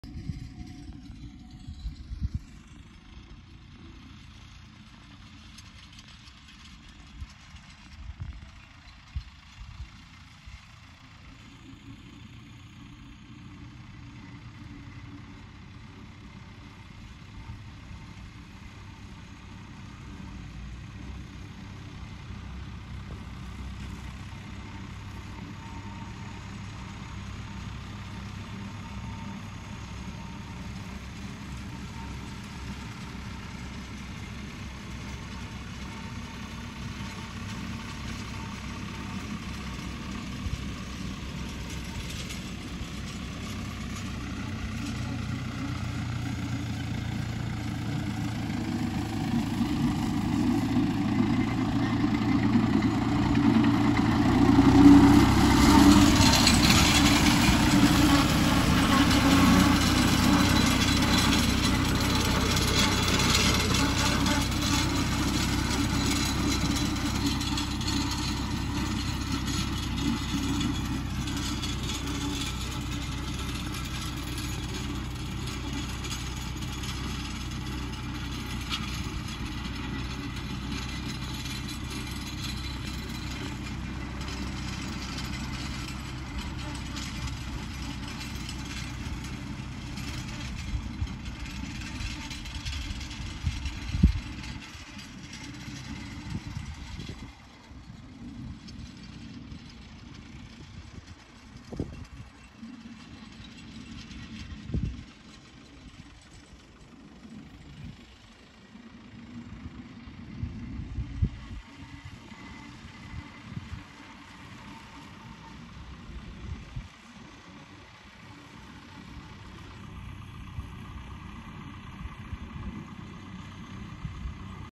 2022 yılı sezonun son yonca biçimleri yapılıyor
Bartın da 2022 yılı son yonca biçimi yapıyoruz ve sezonu kapatıyoruz